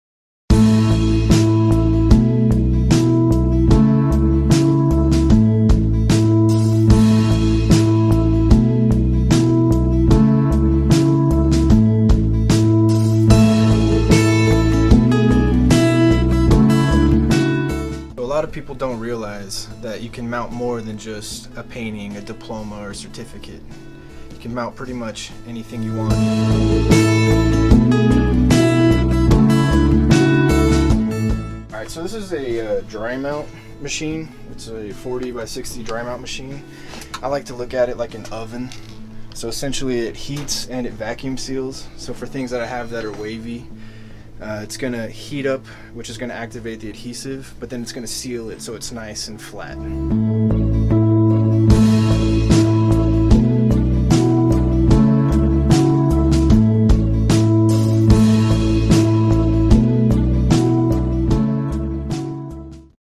Dry Mounting Art - Art Masters Framing El Paso
Dry mounting Is a great option to help preserve any delicate art, photos, fabrics or documents. We’re glad to provide this service right here, in our shop! 🖼